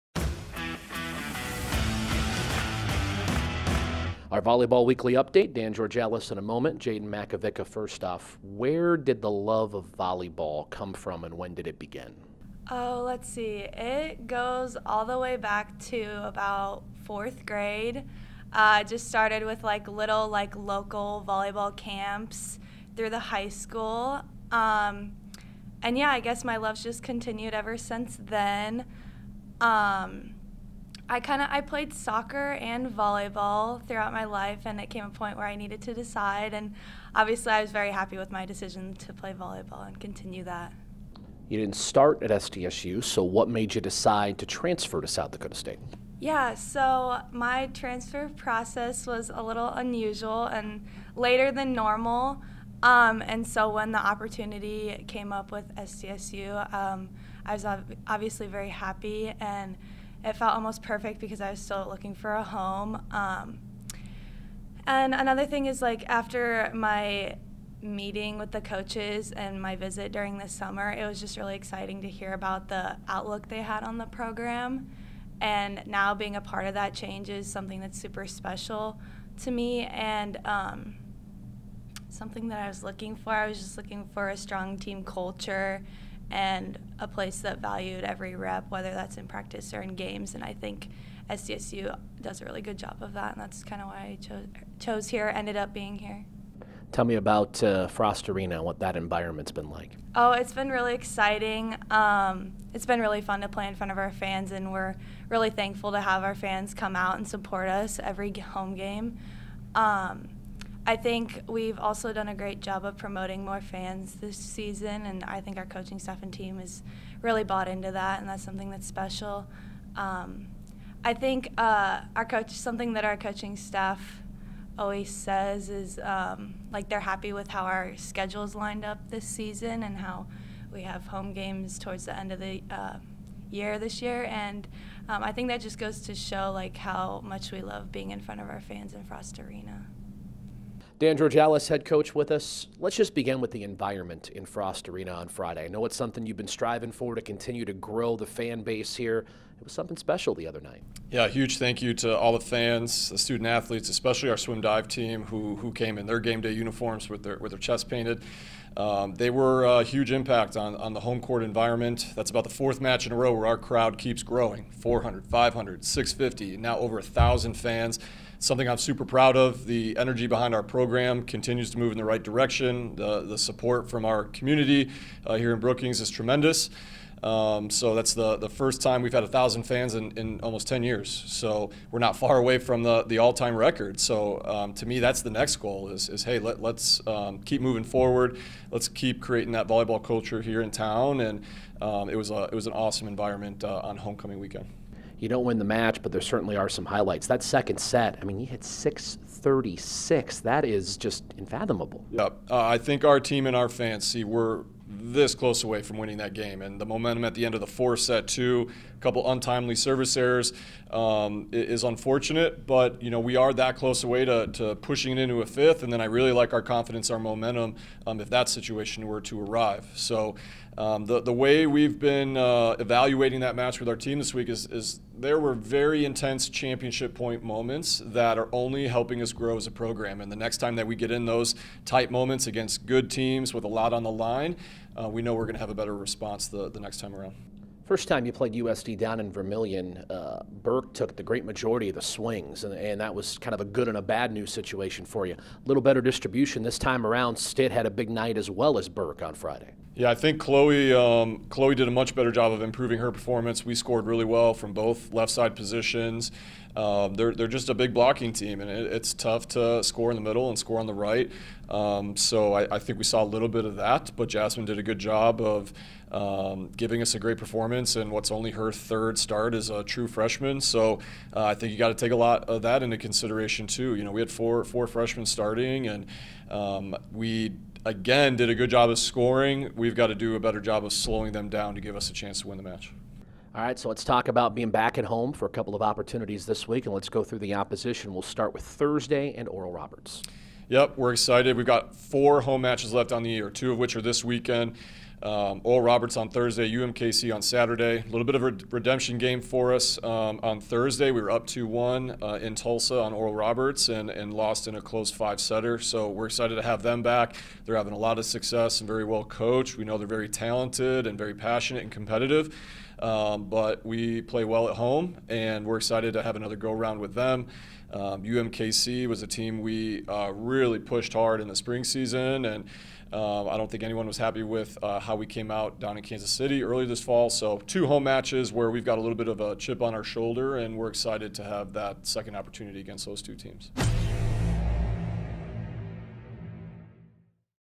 Volleyball Weekly Update (10.26.2021)
Volleyball head coach Dan Georgalas and junior Jadyn Makovicka recap last Friday night's match with South Dakota and preview a pair of home matches with Oral Roberts and Kansas City.